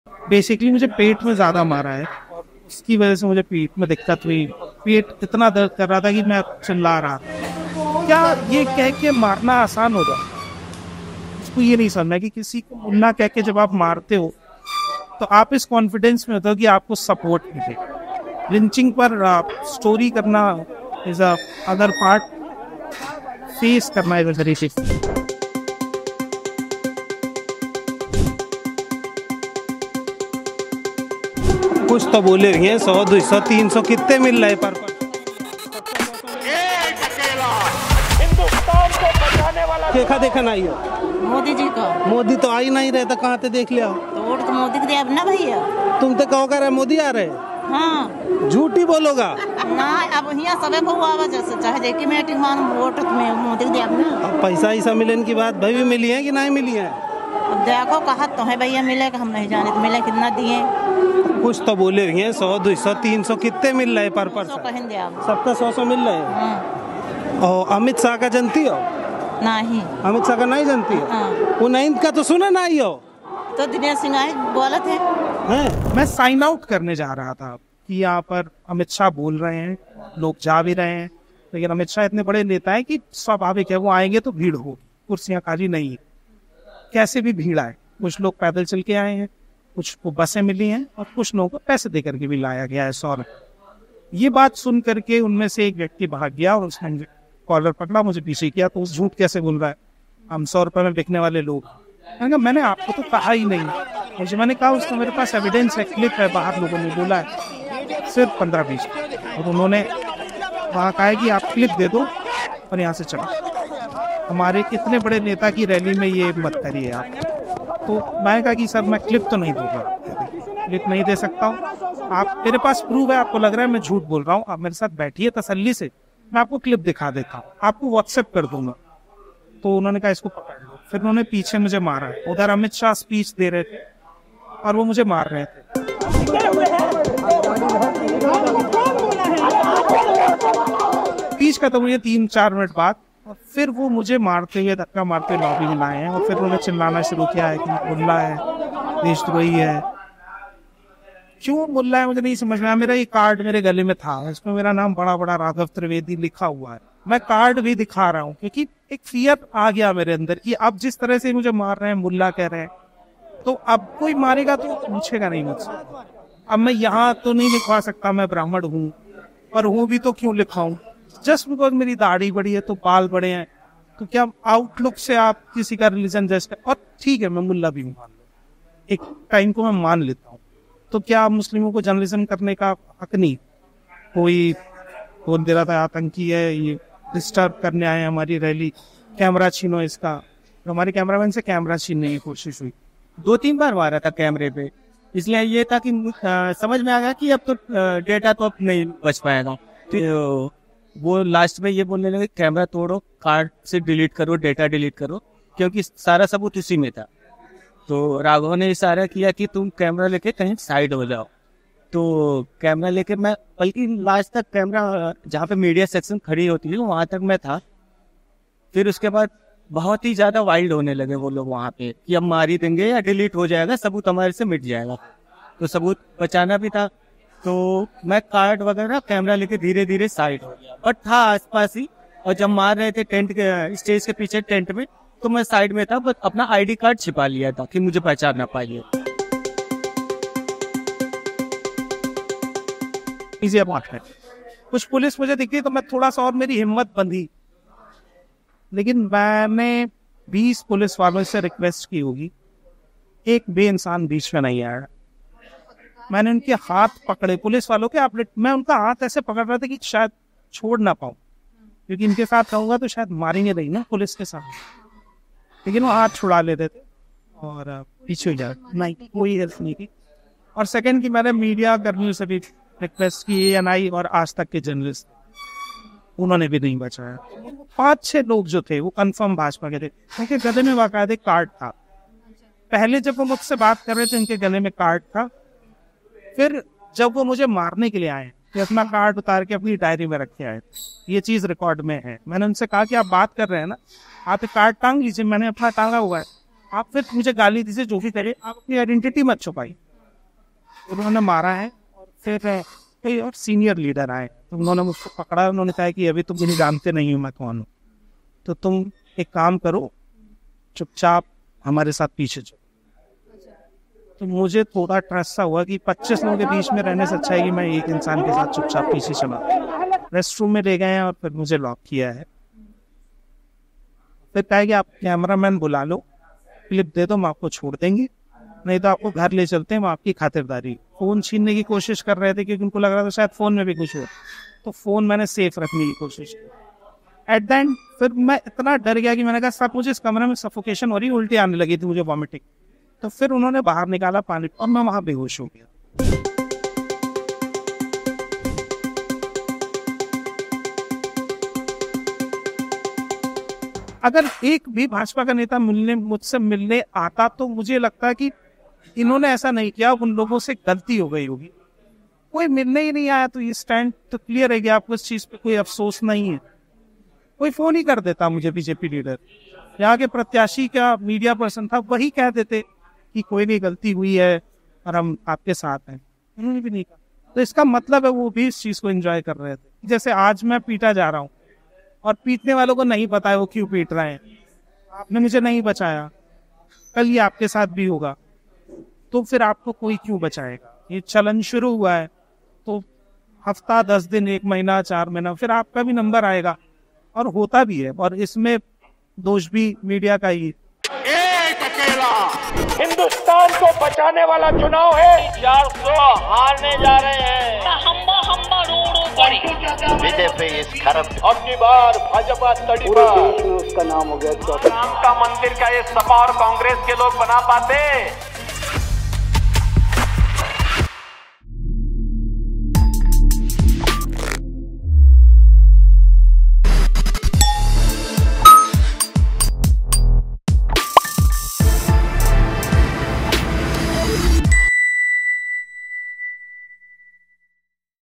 'Wanted My Camera, Thought I'm Muslim': Journalist Beaten At Amit Shah's Rally | The Quint
Raghav Trivedi, a journalist with digital news outlet Molitics was brutally beaten up at a public rally held by BJP's Amit Shah. "They wanted to delete the evidence from my camera, most of the crowd thought I'm Muslim so they joined in to beat me further," recalled Trivedi who was later hospitalised with injuries after he had fainted in the rally. Neither the police nor the media present at the spot helped him, he told The Quint.